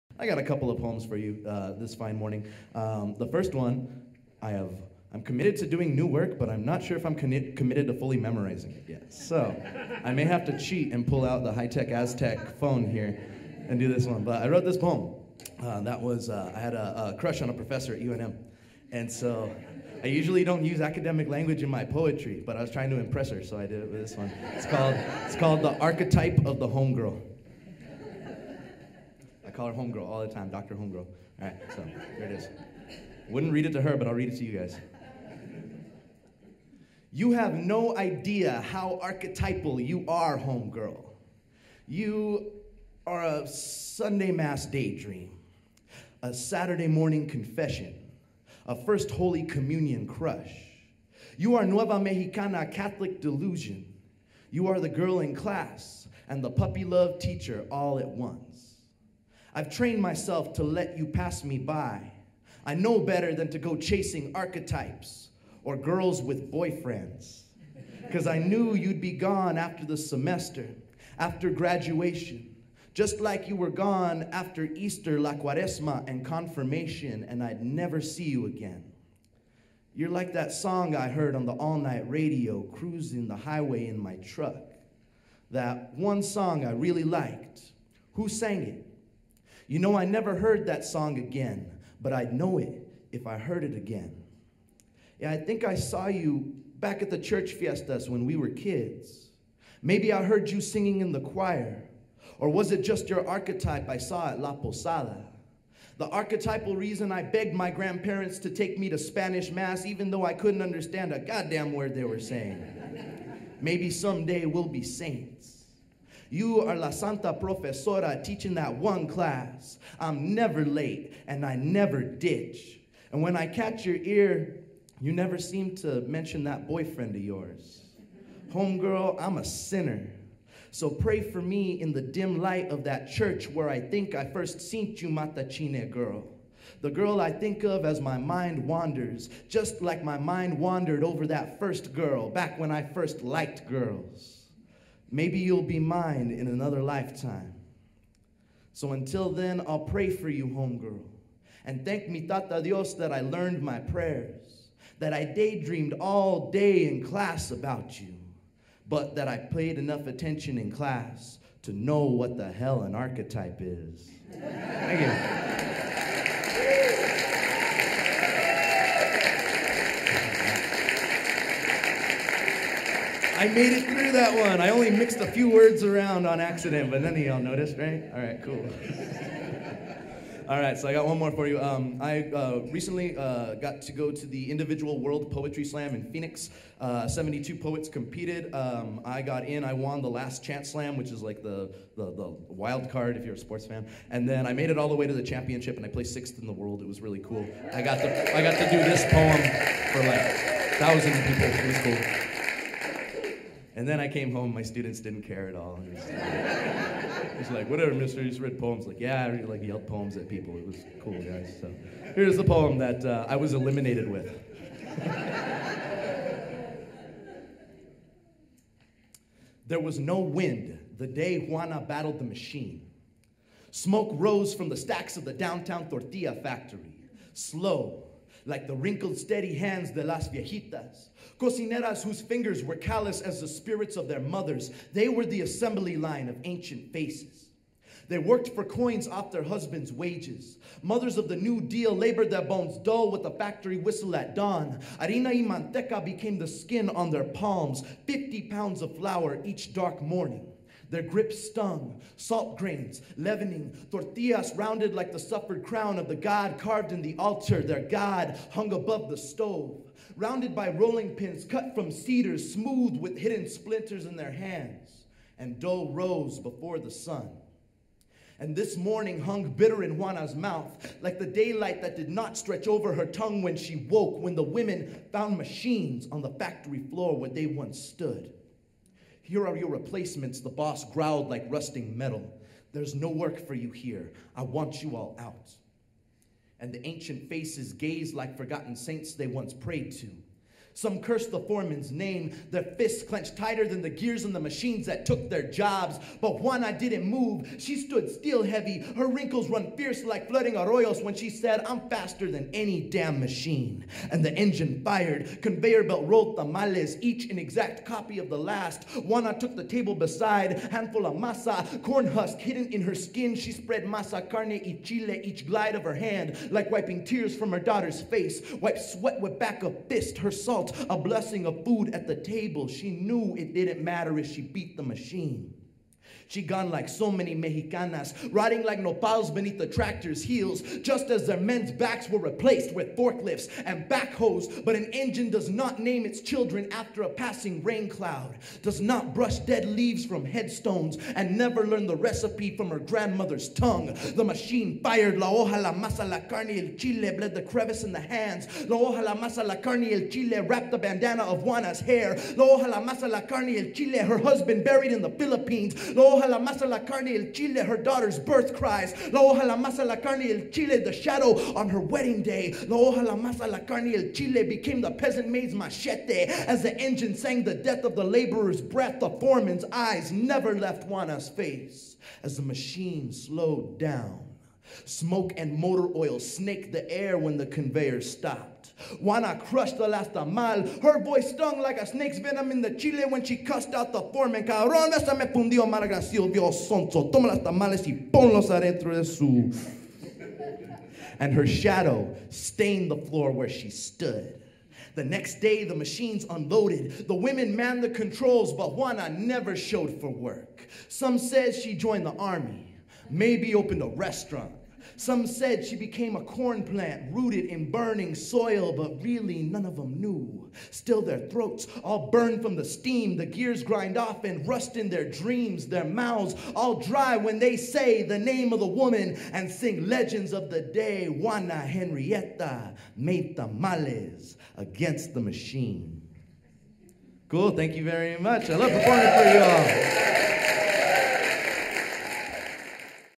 Damien Flores, poet
Damien Flores performs two of his poems:
'Archetype of the Home Girl'  0:00:36.7
'The Day Juana Battled the Machine'  0:03:38.0